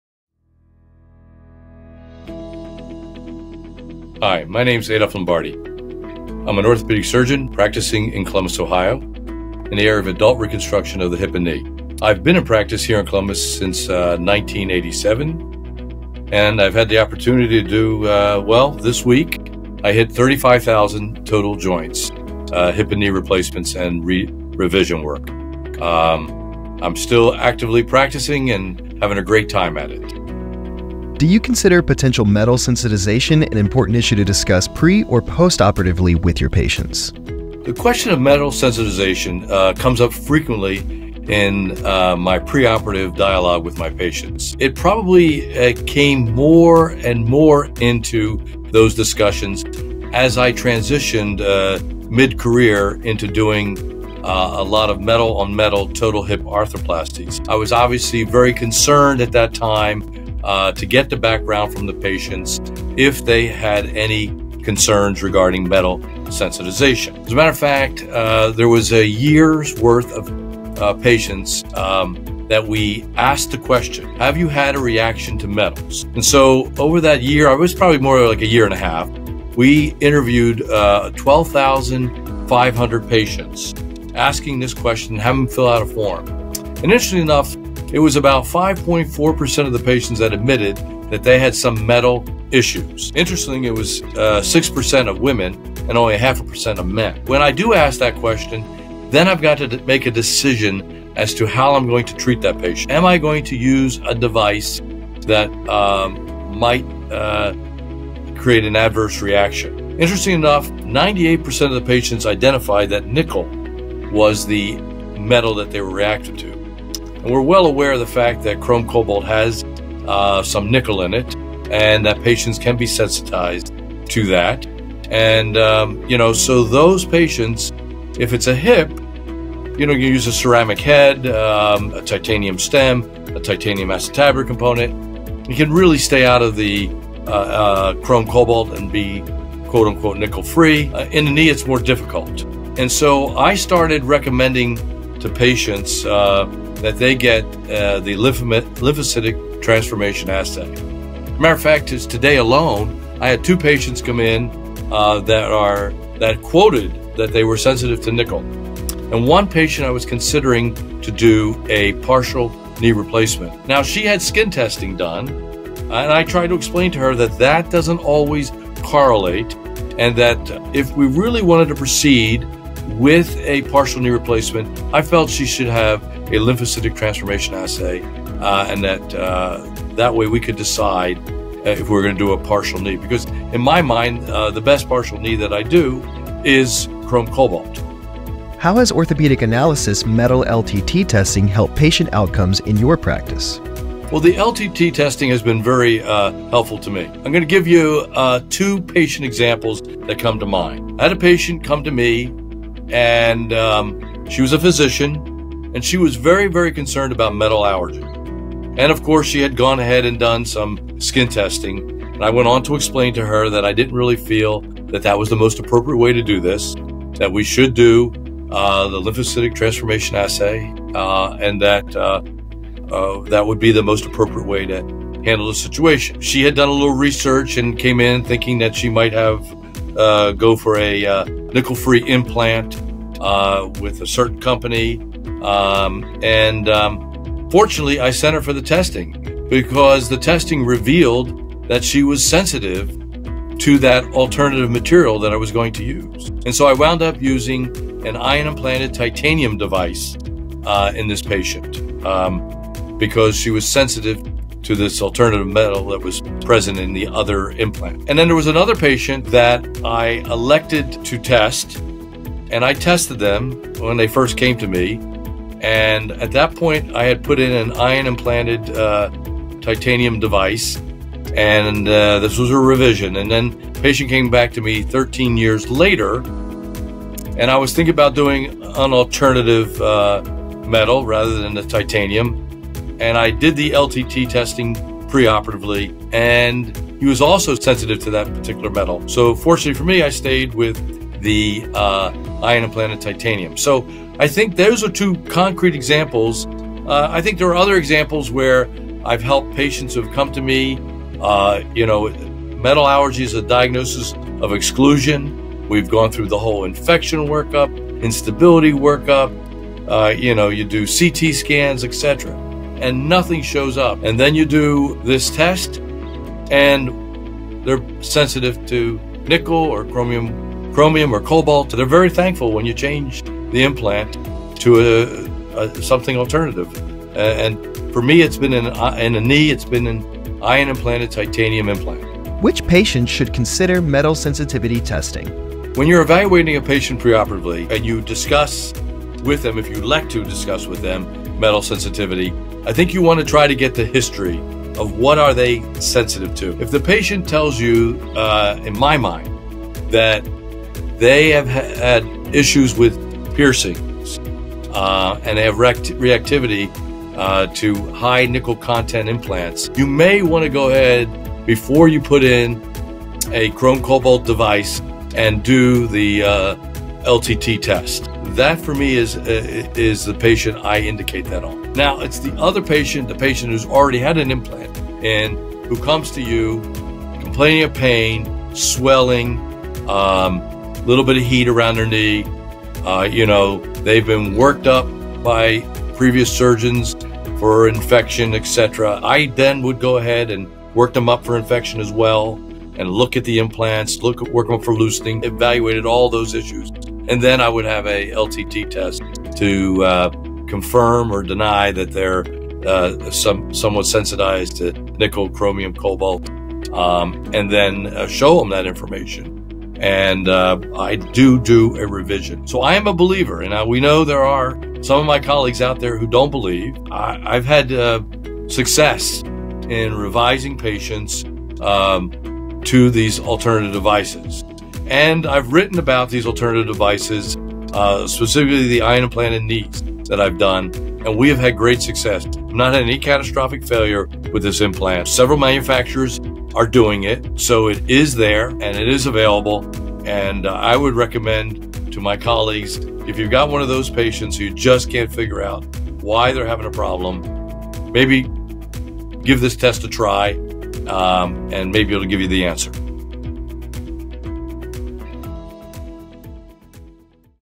The importance of Metal Allergy Testing for Joint Replacement Surgeons (Dr. Adolph Lombardi)
In this video, Dr. Adolph Lombardi (World Renowned Orthopedic Surgeon) discusses the importance of finding out if patients have a sensitivity to metals before joint replacement surgery. Dr. Lombardi also highlights that throughout his own studies, they have found that about 5.4 % of patients asked about metal sensitivity admitted having issues with metals. Dr. Lombardi discusses the efficacy of Orthopedic Analysis' lymphocyte transformation test (metal-LTT) compared to skin patch testing in identifying patients with a metal allergy to better decide what type of material to use in sensitive patients. Dr. Lombardi discusses how patients are thankful once you identify their sensitivities and revise their implant to alternative devices that do not contain offending metals.